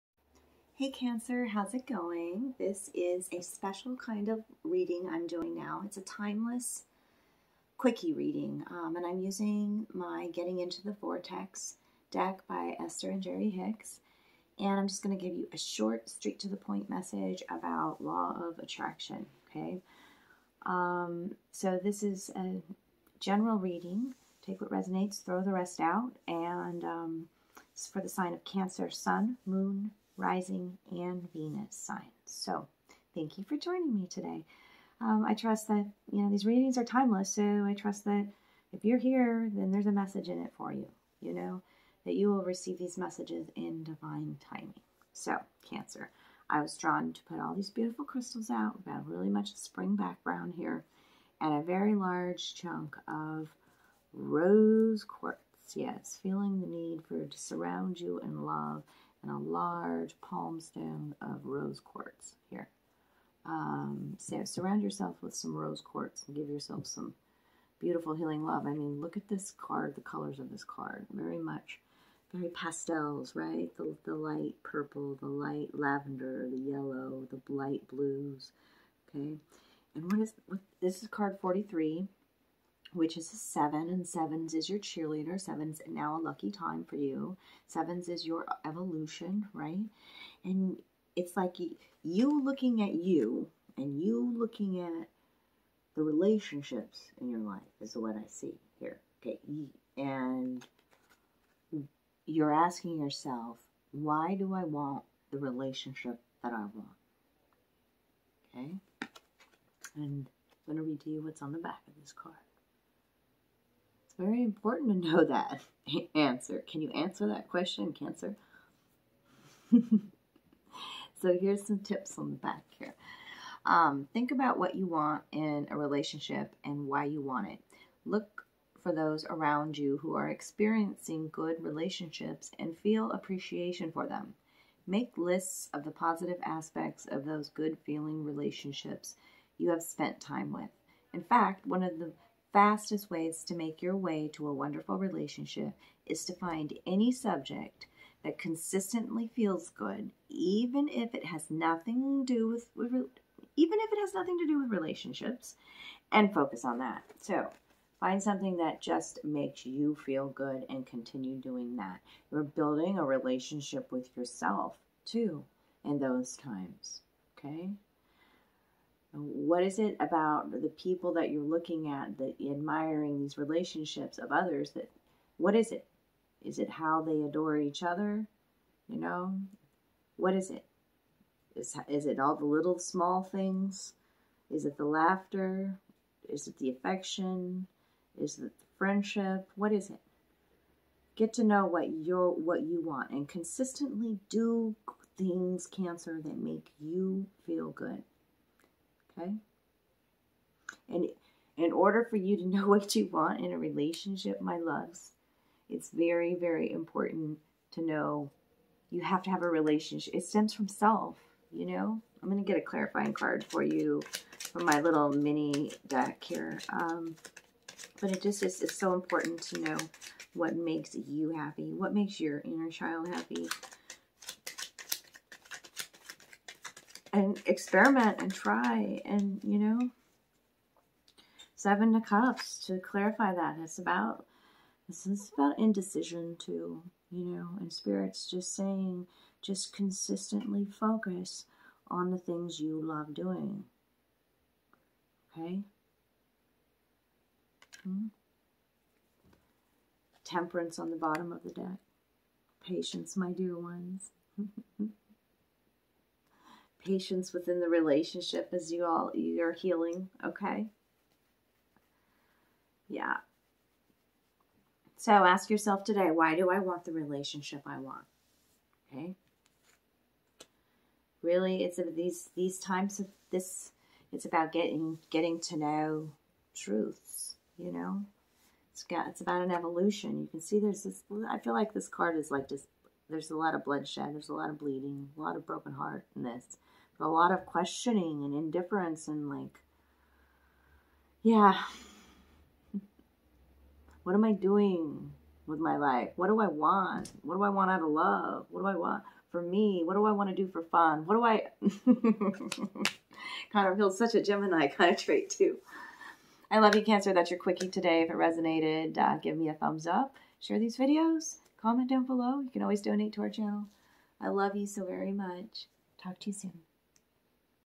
Cancer Timeless Quickie Reading~ What Do I want from this relationship?
This is a special quickie timeless reading for the sign of Cancer, Sun, Moon, Rising and Venus. This is a general reading.
The deck I used today
Getting into the Vortex by Esther and Jerry Hicks

Blessings